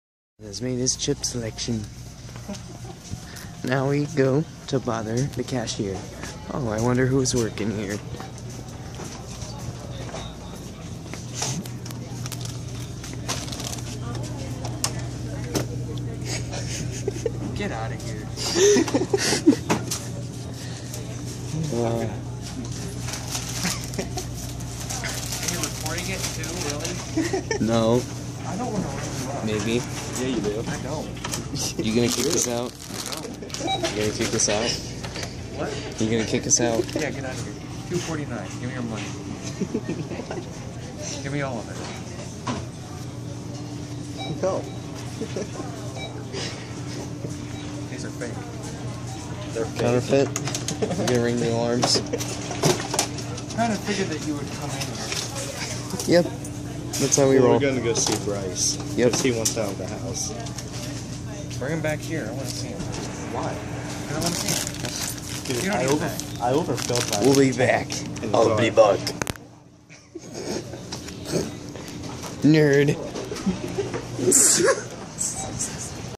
Bothering Brandon at work😂😂
Me and Justin venture to Marc's and bother our band mate Brandon.